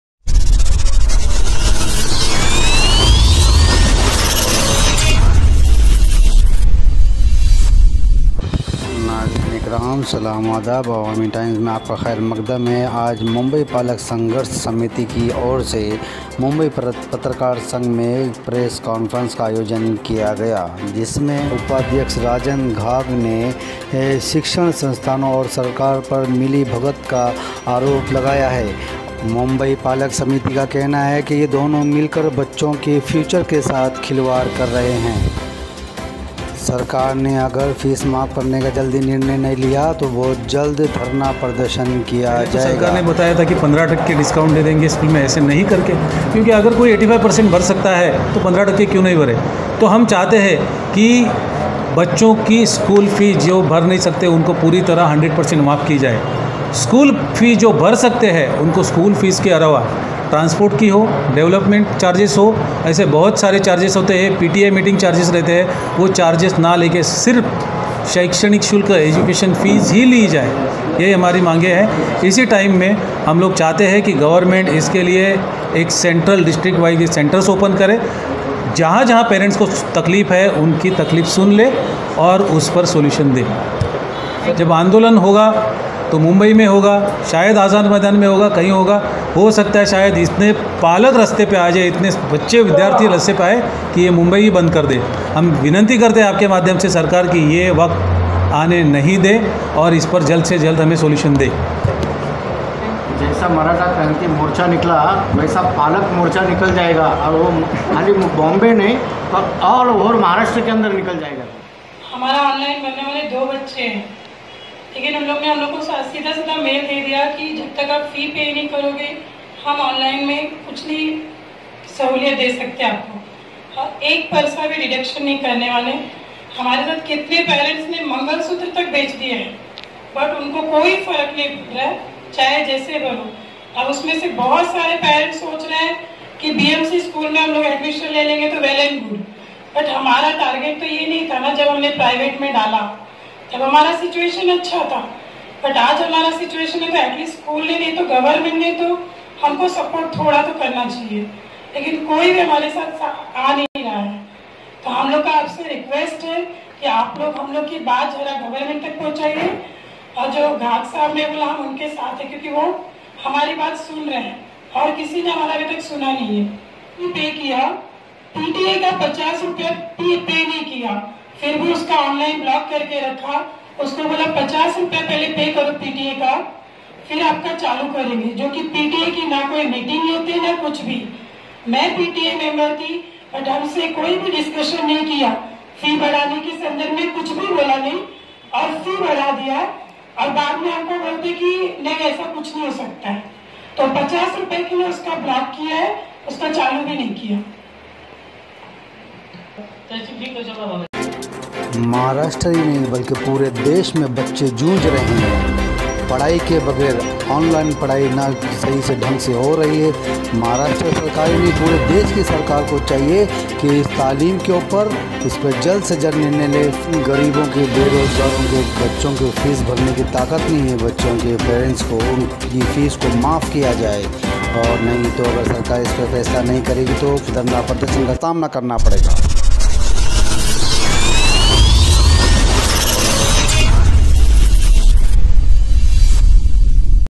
सरकार स्कूल फीस माफ़ की घोषणा नहीं करती,तो जोरदार विरोध होगा: राजन घाग
मुंबई पालक संघर्ष समिति के उपाध्यक्ष  राजन घाग ने प्रेस कॉन्फरन्स में कहा की  यदि सरकार सभी छात्रों के लिए शुल्क माफी की घोषणा नहीं करती है, तो जोरदार विरोध होगा डेढ़ साल से बेरोजगारों के बच्चों की पढ़ाई में बाधा डालने वाले स्कूलों के खिलाफ कार्रवाई हो , अब डेढ़ साल से अधिक समय हो गया है कि covid की महामारी फैल गई है और हजारों नहीं बल्कि लाखों लोगों की नौकरी चली गई है. यह अफ़सोस की बात है कि ऑनलाइन ट्यूशन के नाम पर वे अत्यधिक शुल्क ले रहे हैं और हमारी जानकारी के अनुसार मुंबई में
 अधिकांश स्कुलो ने बच्चोंकी पढाई रोक दी है, जिन्होंने फीस नहीं देने वाले छात्रों की पढ़ाई रोक दी है। हम इसे बर्दाश्त नहीं करेंगे और अब हमें पूरी जानकारी मिल रही है कि कितने अन्य स्कूलों ने अपनी पढ़ाई बंद कर दी है। बच्चों द्वारा फीस का भुगतान न करना।सम्मेलन में पत्रकारों से बात करते हुए, मुंबई पलक संघर्ष समिति के उपाध्यक्ष राजन घाग ने शैक्षणिक संस्थानों और सरकार पर मिलीभगत का आरोप लगाया और कहा कि दोनों छात्र अपने शैक्षणिक करियर के साथ खेल रहे थे। लोगों से बात करते हुए कि लोगों के पास नौकरी नहीं है, `15% फीस माफी की बात करने वाली सरकार मुखिया है। भीख न मांगने से कार ऐसे छात्रों की 2% फीस माफ कर देती ताकि उनका शैक्षणिक वर्ष खराब न हो। "जब भी बाढ़ आती है," उन्होंने कहा जब भूकंप आता है, तो सरकार उसके लिए हजारों करोड़ रुपये प्रदान करती है, लेकिन शिक्षा के मामले में सरकार ने आंखें मूंद ली हैं, जबकि सरकार को छात्रों की फीस  माफ करनी  चाहिए। वह भविष्य में फीस का भुगतान करेगा। उन्हें भी समय देना चाहिए। उन्होंने सरकार की आंखों पर पट्टी बांधकर शिक्षा पर नाखुशी जाहिर करते हुए कहा कि अगर सरकार आठ-दस दिन में कोई ठोस फैसला नहीं लेती है तो हमारी कोई मांग नहीं है। पत्रकारों से बात करते हुए प्रफुल्ल पवार (महाराष्ट्र) प्रवक्ता) ने कहा, "जब हम सरकारी अधिकारियों से शुल्क माफी के बारे में बात करते हैं, तो वे राजनीतिक नेताओं की तरह प्रतिक्रिया देते हैं।" हम देख रहे हैं और हम कर रहे हैं। संघर्ष समिति और संरक्षक दोनों इस जवाब से संतुष्ट नहीं हैं। हम इसका कड़ा विरोध करेंगे। . प्रेस कांफ्रेंस में एक महिला अभिभावक ने भी सरकार की शिक्षा नीति पर कड़ी आपत्ति जताते हुए कहा कि सरकार को उन लोगों के बारे में सोचना चाहिए जिनके पास फिलहाल नौकरी नहीं है, इसलिए वह सभी बच्चों की फीस  माफ कर दें.